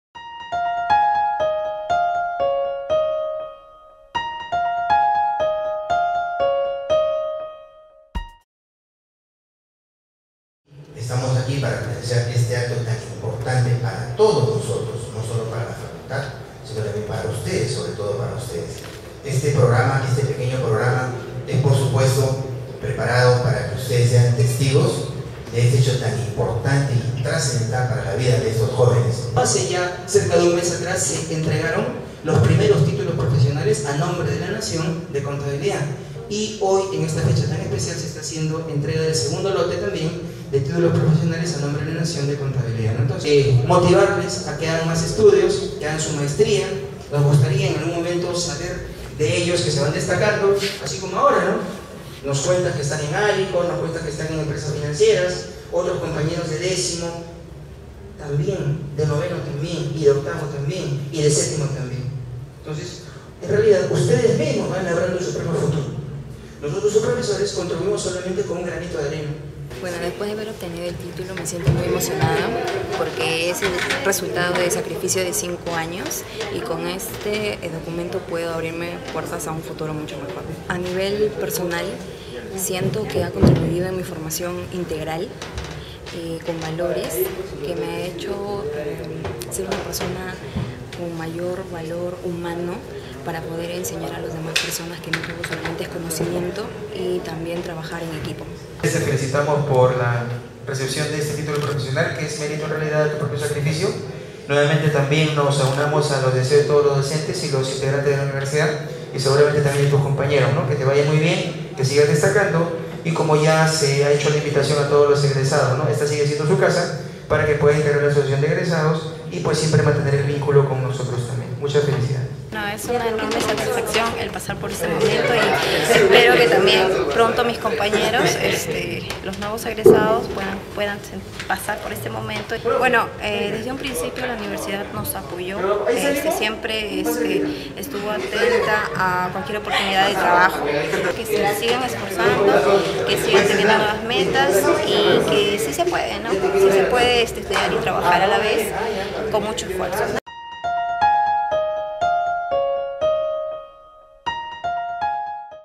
Ceremonia de Titulación de Contabilidad - UCH 2014
La Universidad de Ciencias y Humanidades (UCH) entregó títulos de licenciatura a un nuevo grupo de bachilleres de Contabilidad con mención en Finanzas, el pasado 14 de mayo, en mérito a su esfuerzo y dedicación durante cinco años de carrera.